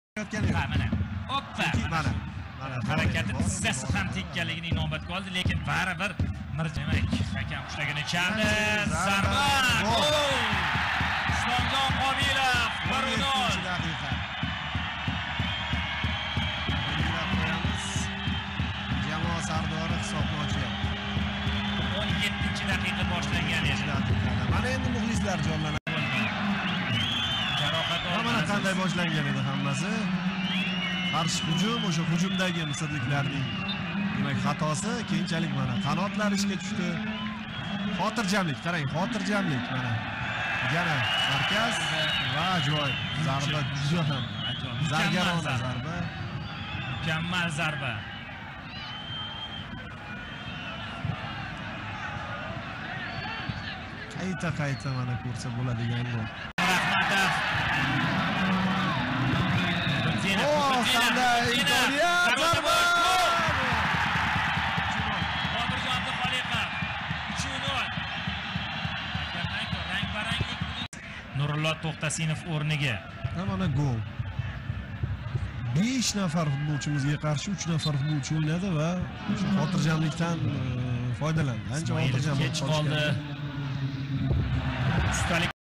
🏆 ЎРТОҚЛИК УЧРАШУВИ 🇺🇿 ЎЗБЕКИСТОН 🆚 МИСР 🇪🇬 3:2
Uchrashuv Yakunlandi.
Bugungi O'rtoqlik uchrashuvi da
"O'zbekiston-OTJ" Namangan da "Misr-OTJ" jamoasini 3:2 hisobida g'alabaga erishdi.

Namanganlik muhlislarimmiz bugungi o'yindan hursand. Yoshlarimmizni qo'llab-quvatlab turganiz uchun katta raxmat. Olg'a Uzbekiston Olg'a Navbahor.